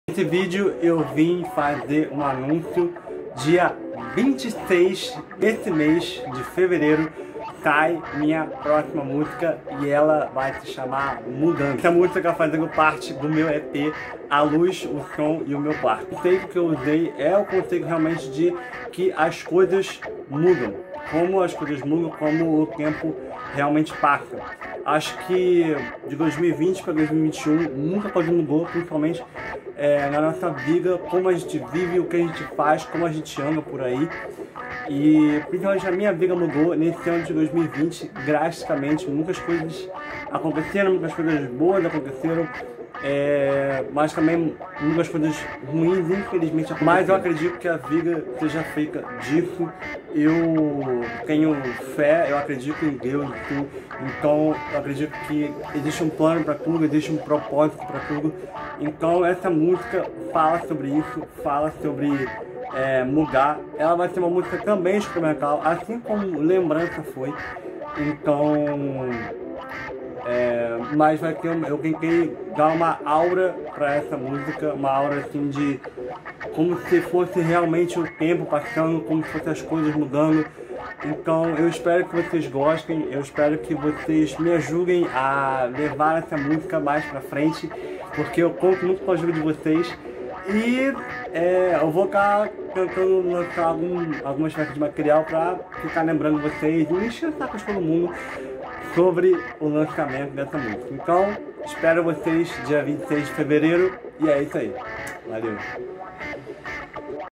Preparem-se para a mudança
Eu disse que era de 2020 para 2021, mas na verdade é 2019 para 2020.
'Mudança' estará no meu Instagram e aqui no canal do Youtube no dia 26 de fevereiro.